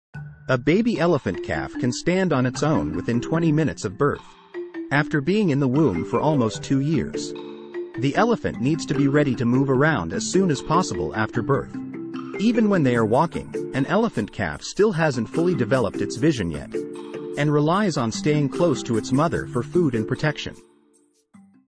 Elephants 🐘 A Baby Elephant's First Steps
Elephant calves are some of the fastest learners in the animal kingdom , learning to walk in just a few minutes after being born!